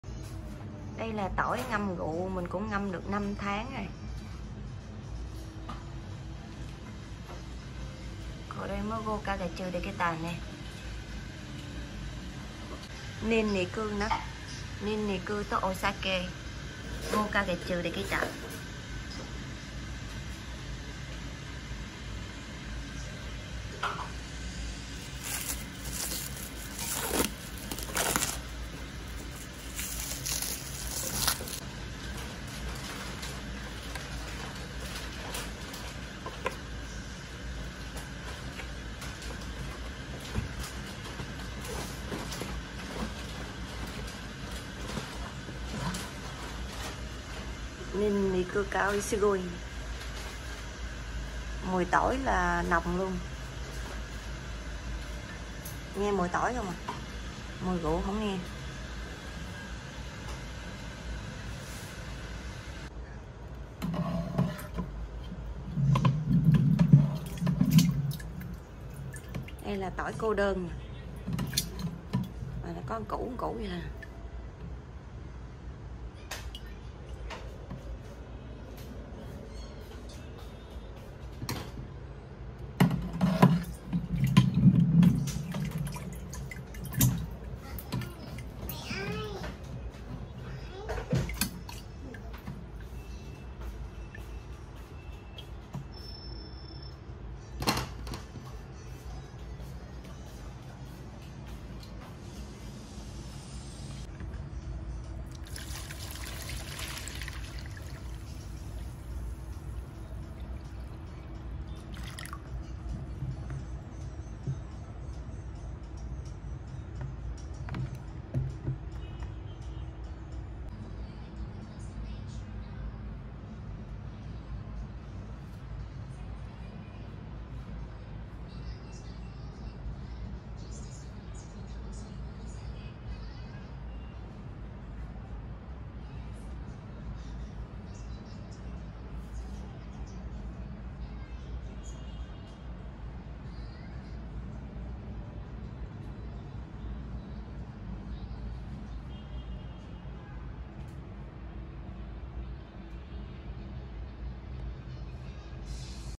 Rượu tỏi cô đơn ngâm ( đã được 5 tháng ) / garlic with alcohol ( 5 months already )
— xem video làm rượu tỏi cô đơn ngâm tại nhà / how to make the garlic with alcohol